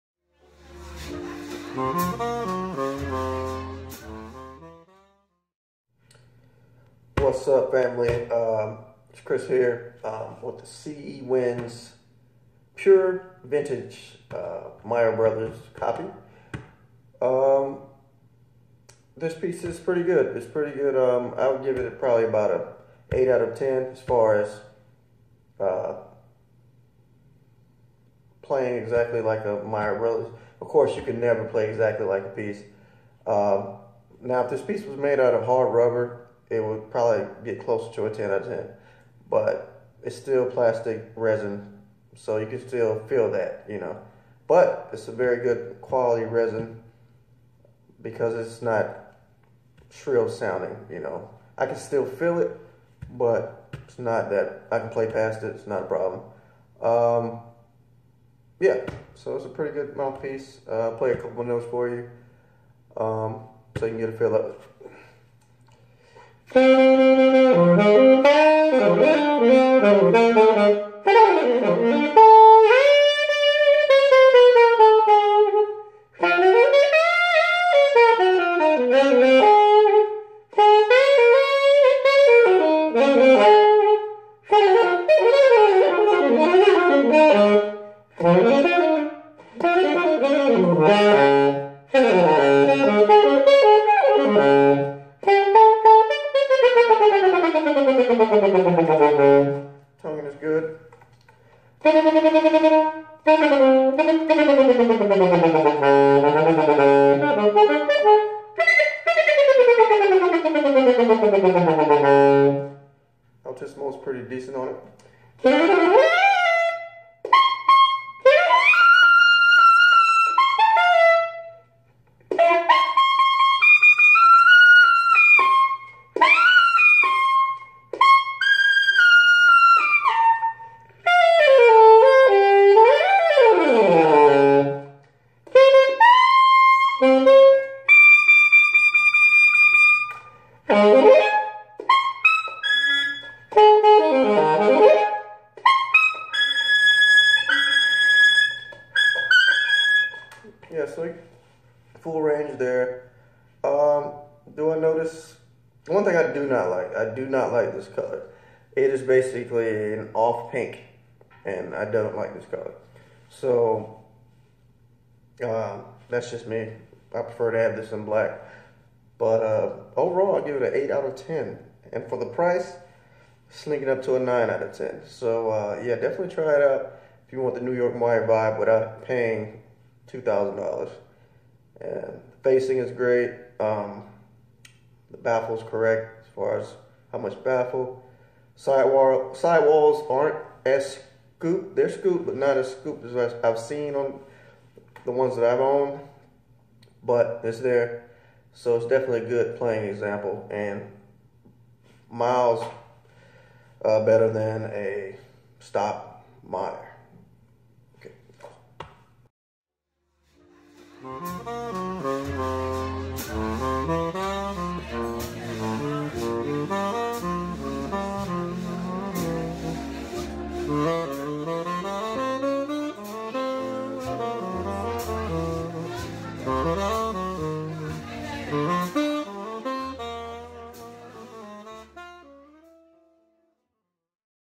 Chris Andrews on CE Winds Pure Vintage Mbros (meyer bros) alto sax mouthpiece King Super 20
More about CE Winds at cewinds.com